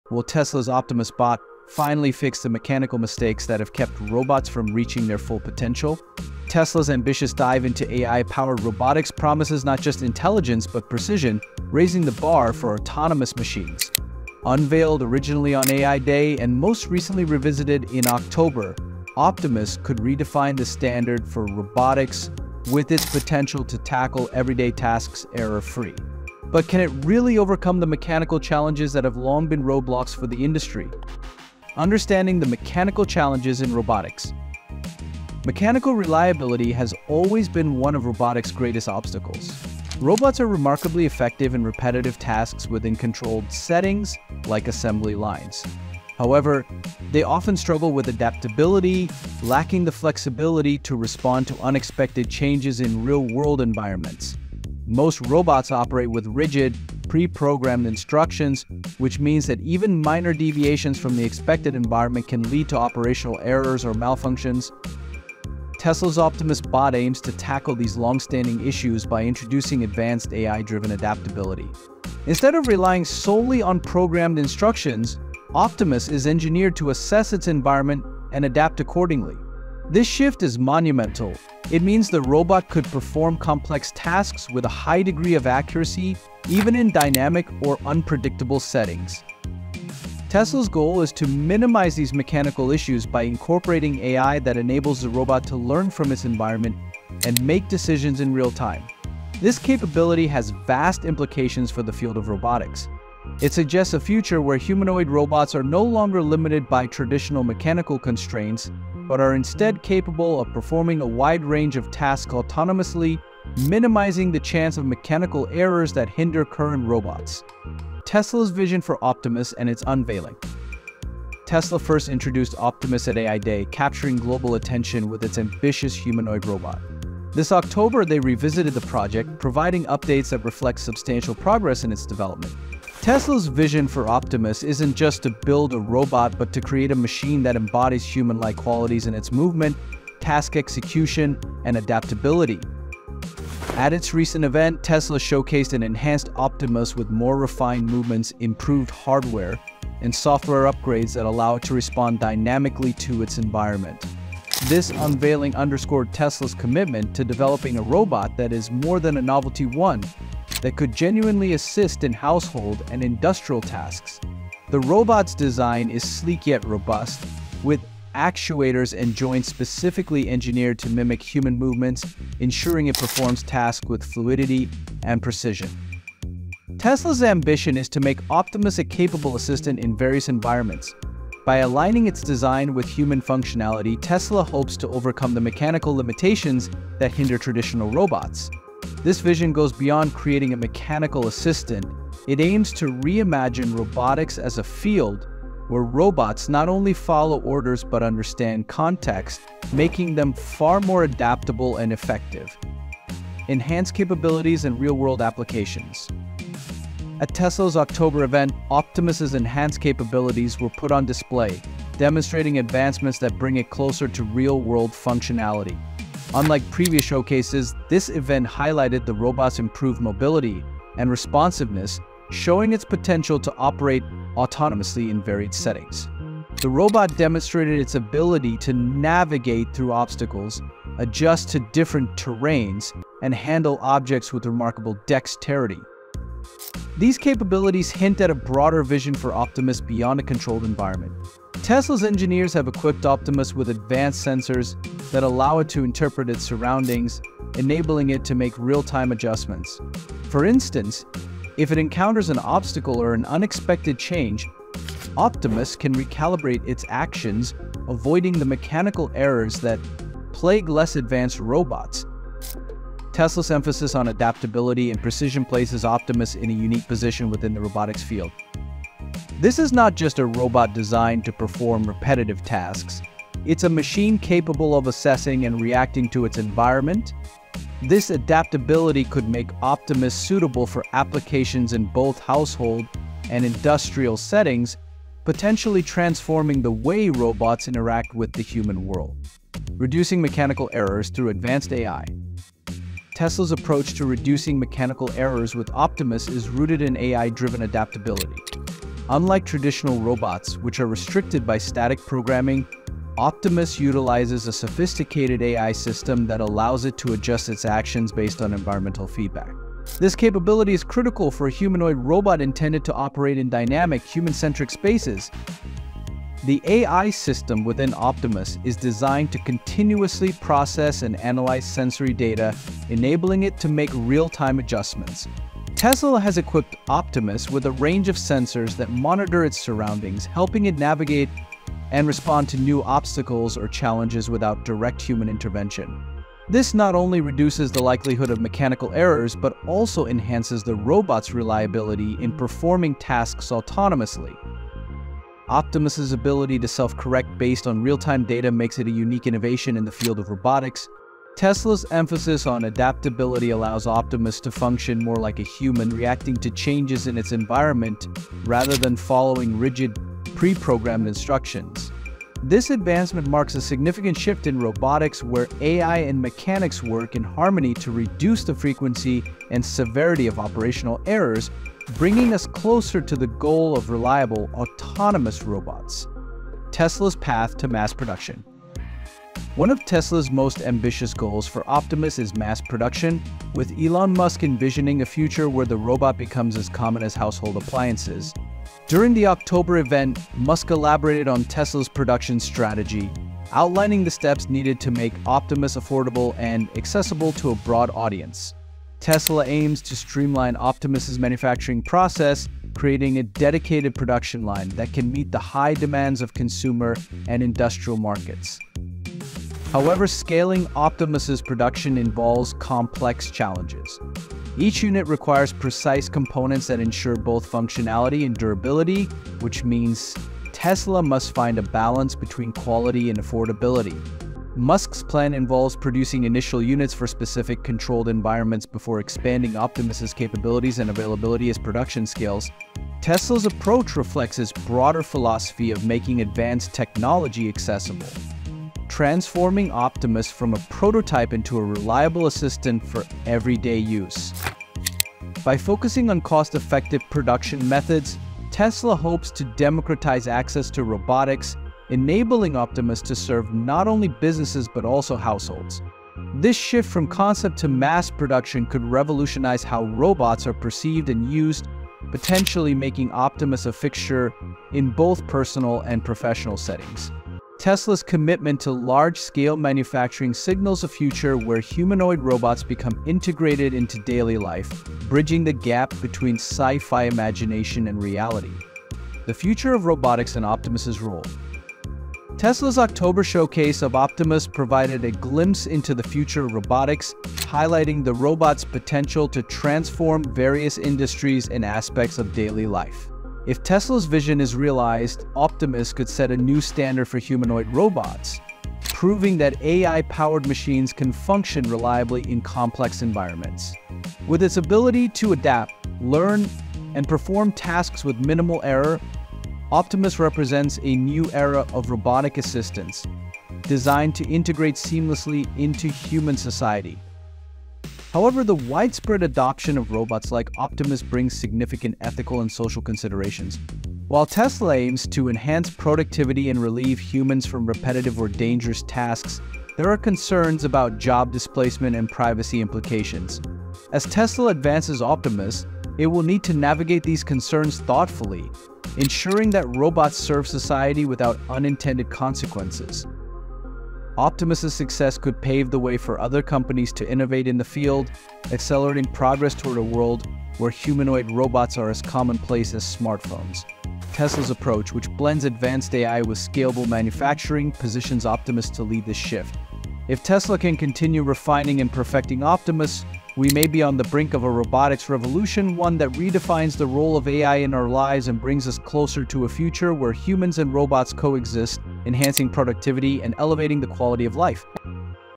Is Tesla's Optimus the END of Robot Mistakes for GOOD?
Robots have ALWAYS struggled with mechanical errors...until NOW! Tesla's Optimus Bot uses cutting-edge AI to adapt and learn, potentially eliminating the mistakes that have plagued robots for decades.

This video dives deep into:

* The history of mechanical failures in robots
* How Tesla's Optimus is DIFFERENT
* Optimus's advanced AI and how it prevents errors
* Real-world applications where Optimus could shine
* The future of robots with fewer mistakes!

Don't miss out on this look at the groundbreaking technology that could change robotics FOREVER!